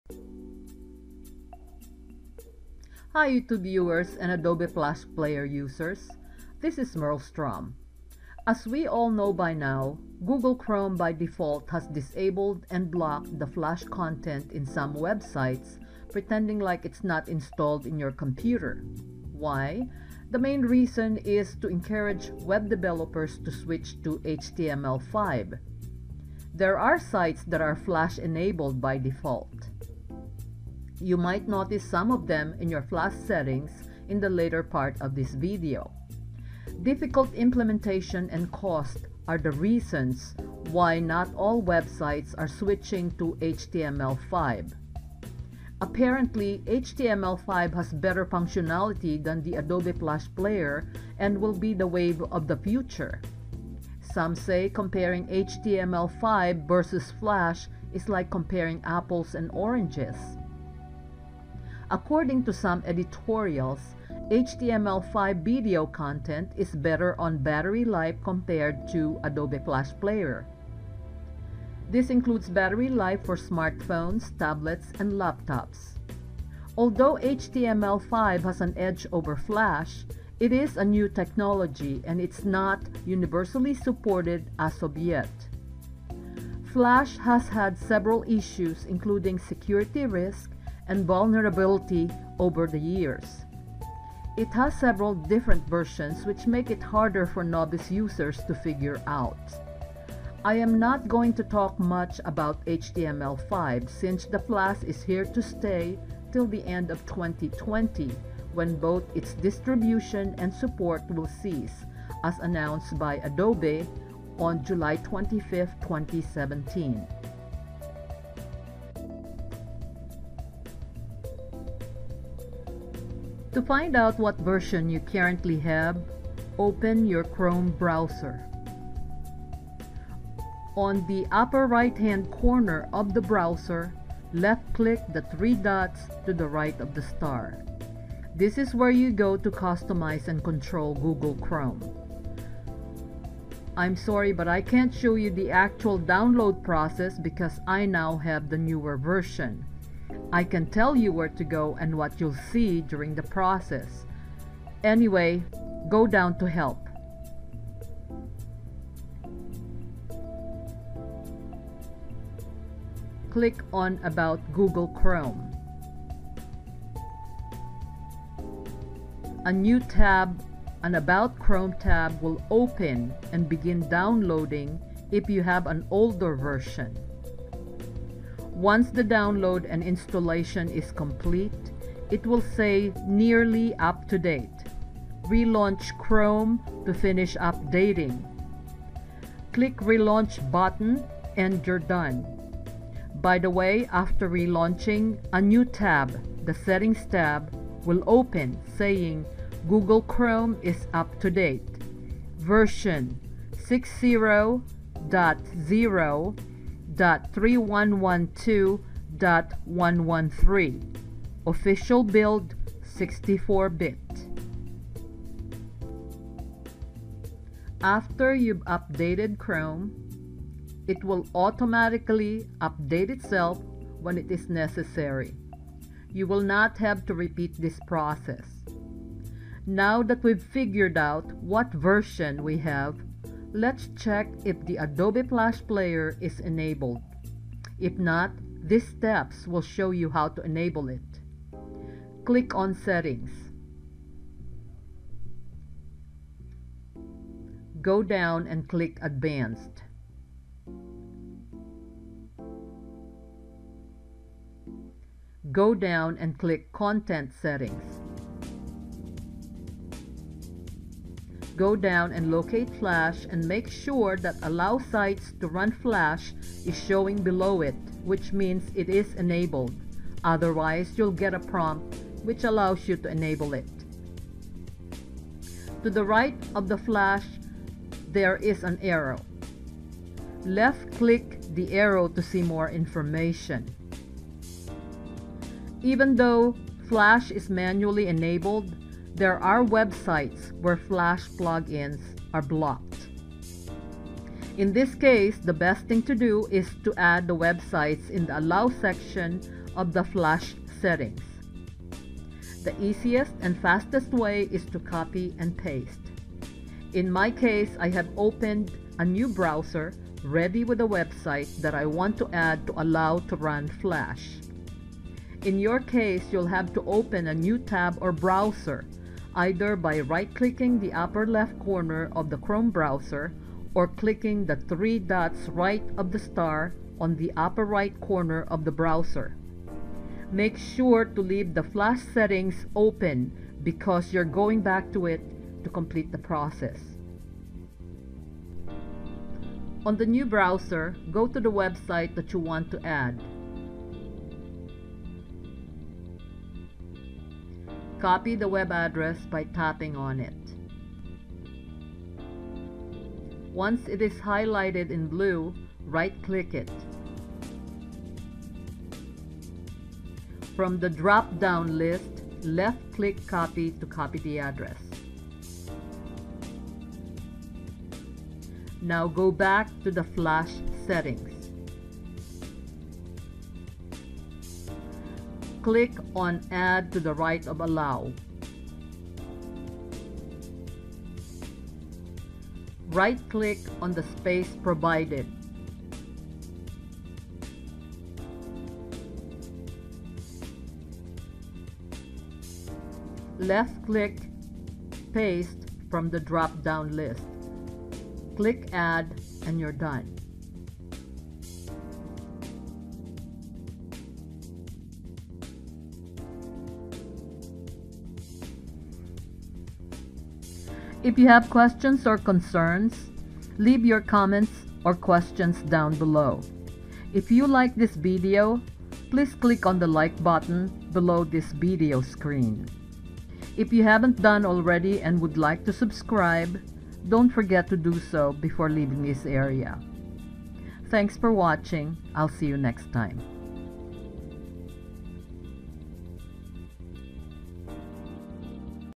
New Fix For Disabled & Blocked Adobe Flash Player For Google Chrome New Version Users (2017)
On April 11, 2017, I posted a video on “How To Enable Adobe Flash Player For Chrome Browser Users”.  The video has gathered a lot of views and comments from YouTube Viewers because this problem is widespread. If you noticed that the Google Chrome Browser that I used in that “How To” video is different than what you are using, most likely you have the latest version. Whether it is an Older or Newer Version of Google Chrome, the procedures are the same. Now, to those of you who are still using the Older Version, this video will show you how to manually update your browser to the latest one.